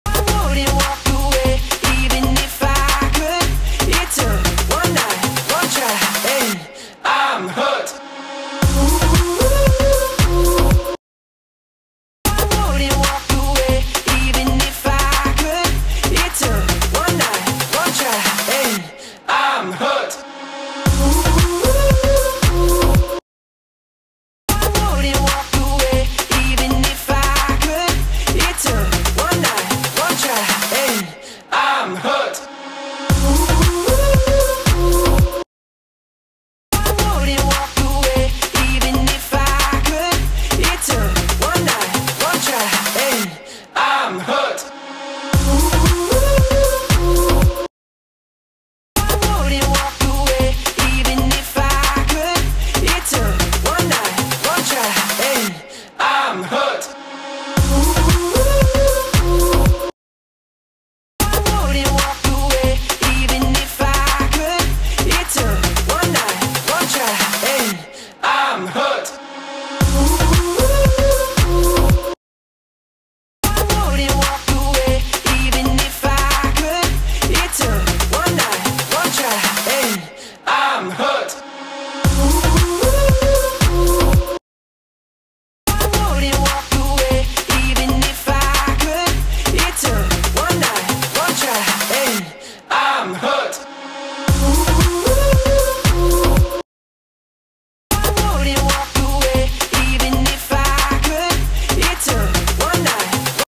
🦕 How I Drew my Dinosaur Sticker 🦕
How I draw a dinosaur in sticker form!! 🦕

Sorry about the music repeating itself, it’s the only audio I could access